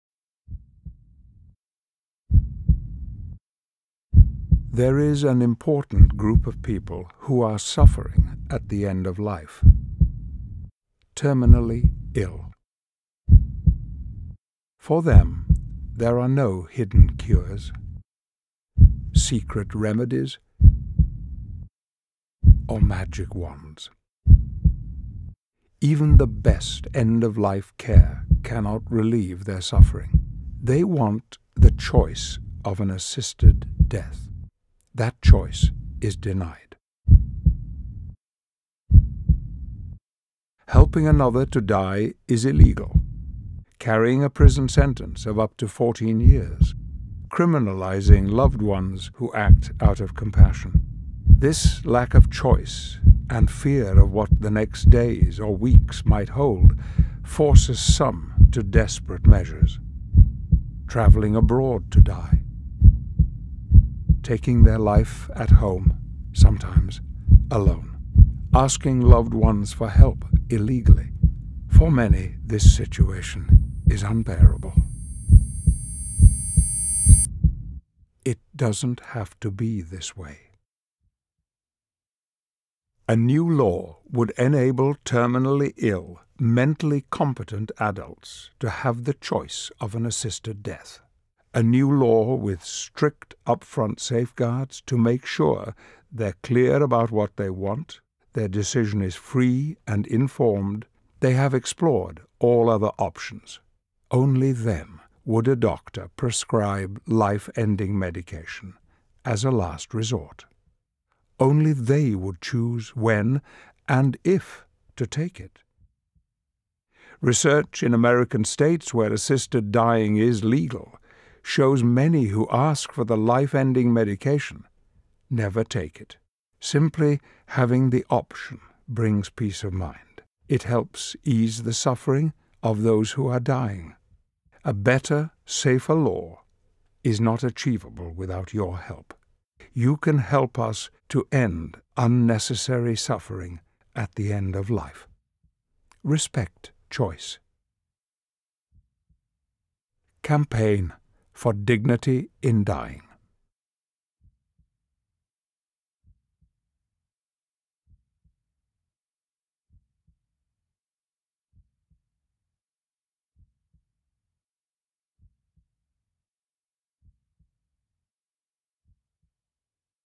Dignity in Dying — Respect Choice
Dignity in Dying Patron Patrick Stewart urges you to join the campaign.